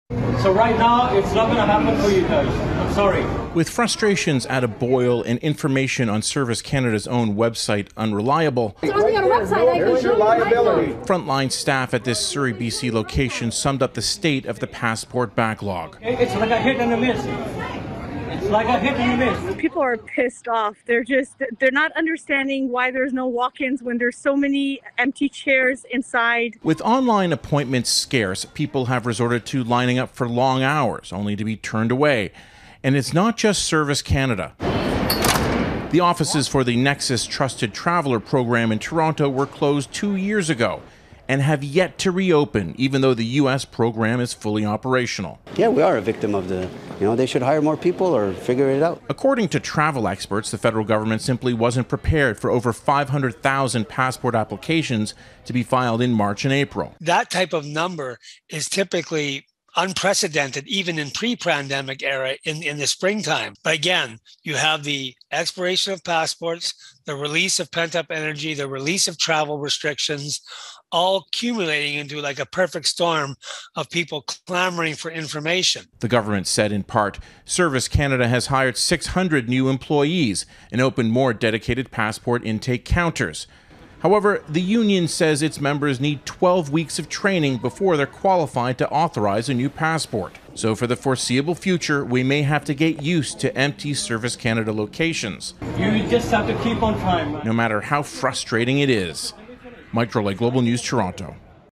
More long lines for Canadians at passport offices
It's become a common sight at Service Canada offices: long lines of people trying to get or renew their passports as the need to travel resurfaces.

But even in May, online appointments remain scarce and the line ups are still as long as ever - with no change on the horizon. Some travel experts say the government was also not prepared for thousands of passport applications to be filed in March and April.

Mike Drolet reports why speeding up the process is not as simple as just hiring more staff.